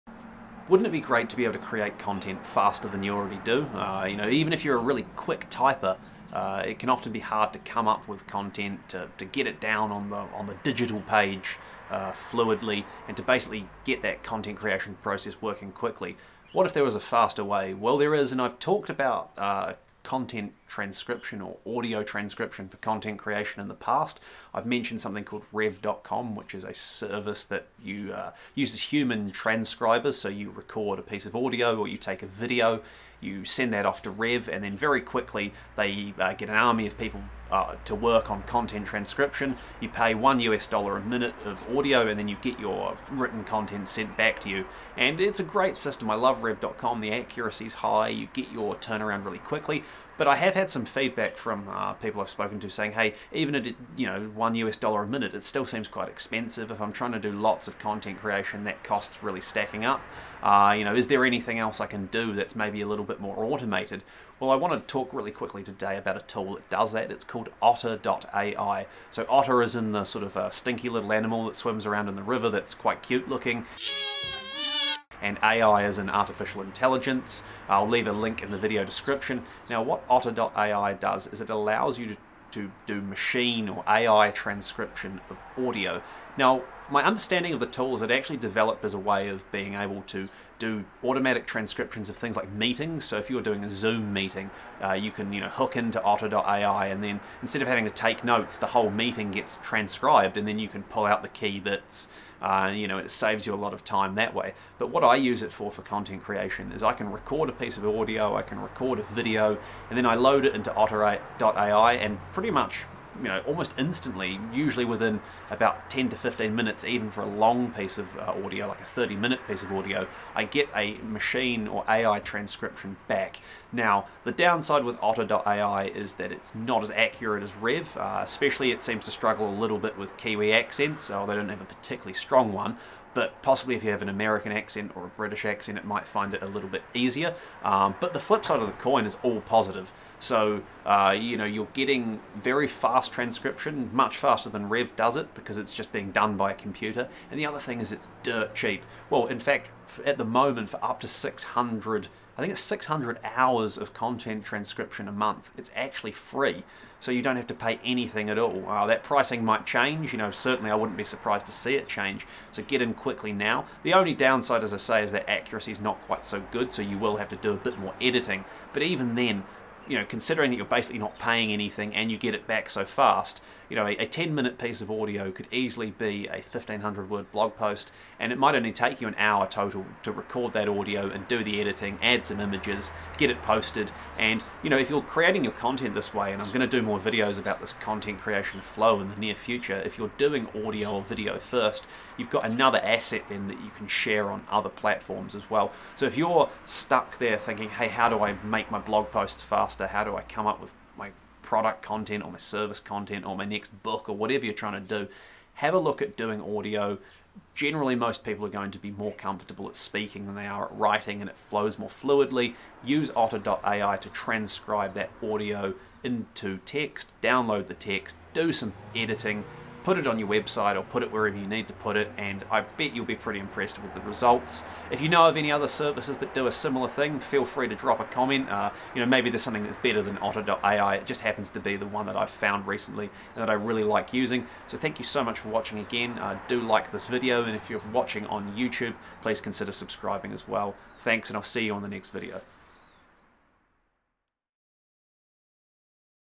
Otter.ai Review - The Secret To Fast Content Creation?
What's the secret to fast content creation?

Here's a hint - it sits underneath your nose and above your chin. That's right, I'm talking about your mouth.

Keep watching this Otter.ai review to discover how to use your mouth to make content creation faster and more effective than you've ever seen before.

I've discussed the value and effectiveness of using voice to text transcription for content creation in the past, and in today's video I'm back with another guide on how you can create more content more quickly using audio transcription.

I recently stumbled across a great tool called Otter.ai, and in this Otter.ai review I cover the pros and cons of using this automatic audio transcription tool versus a human transcription service like Rev.com

I hope you enjoy my review of Otter.ai and find it useful!